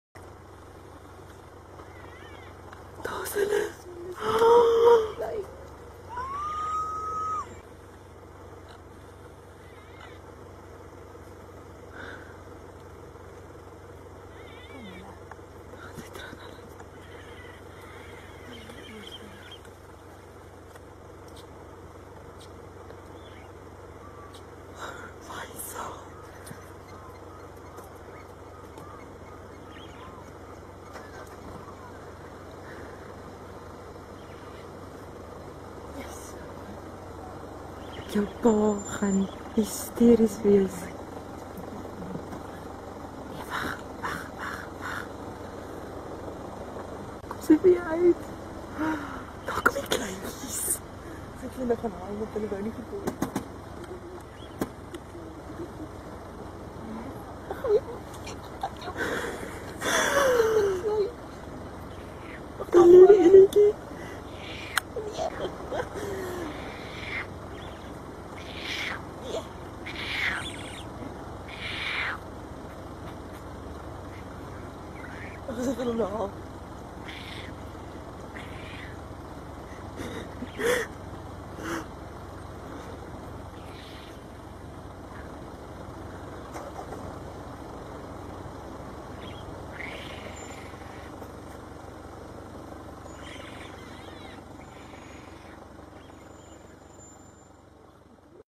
എത്ര വലിയവൻ ആയാലും മക്കൾ എന്നും മക്കൾ തന്നെയാ.. പുലി കുട്ടിയെ നോക്കുന്നത് കാണണോ 😵 Leopard family
Leopard helping it's kids to cross road